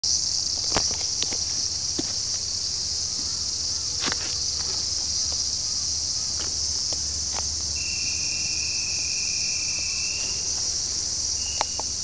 Aeshna juncea j. - paret ova
Aeshna juncea juncea (Linnaeus,1758); paret ova 30.viii.2005 Fucushima-shi Fucusima Japonia.